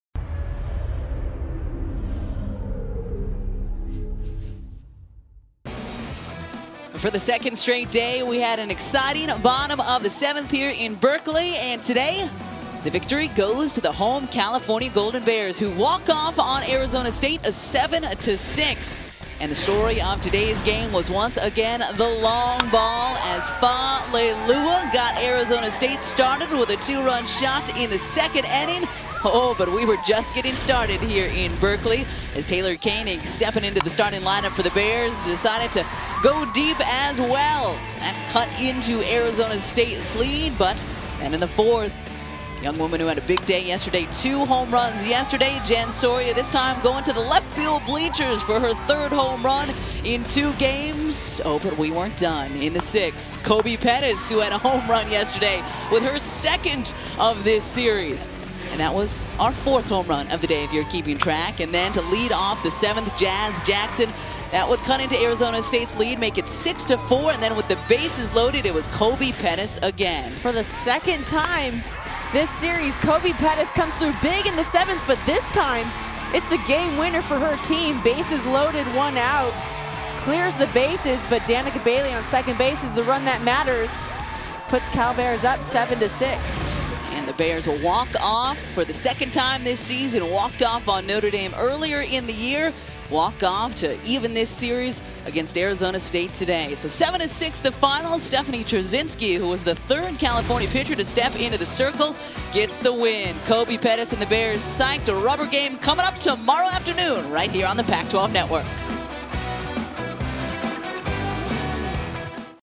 Recap: Cal softball wins walkoff thriller against Arizona State
Pac-12 Networks' Kate Scott and Amanda Freed discuss Cal's thrilling 7-6 walkoff victory against Arizona State Saturday in Berkeley.